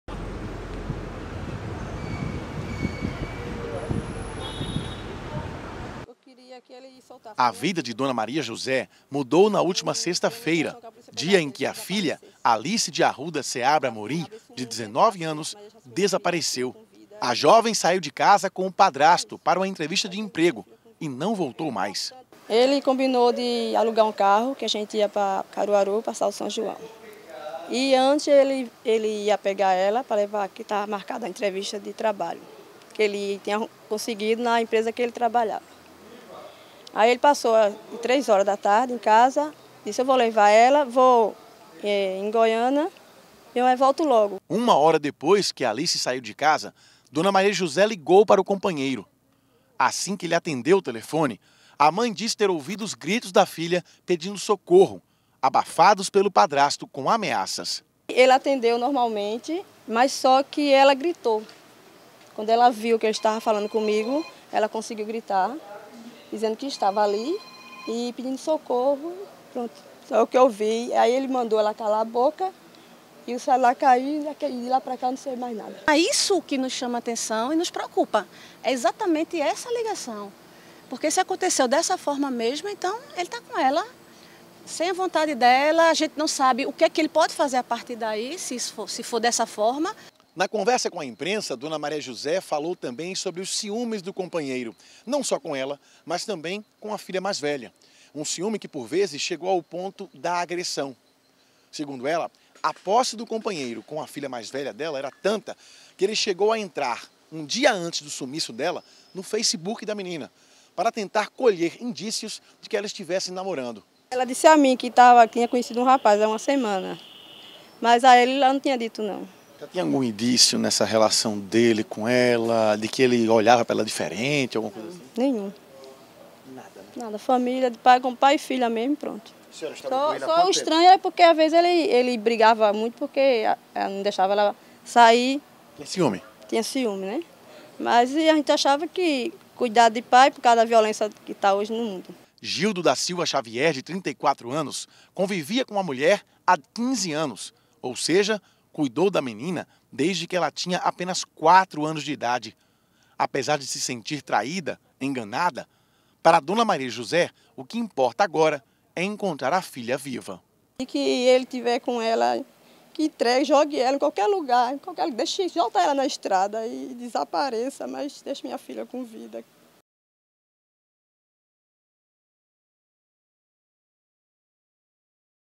Jovem de 19 anos está desaparecida
Maria Alice saiu de casa com o padrasto Gildo da Silva Xavier, 34 anos. Ele foi buscá-la em casa, na Estância, para fazer uma entrevista de emprego em um escritório em Gravatá. Desde a sexta (19) não se tem notícia dos dois. O padastro é o principal sujeito.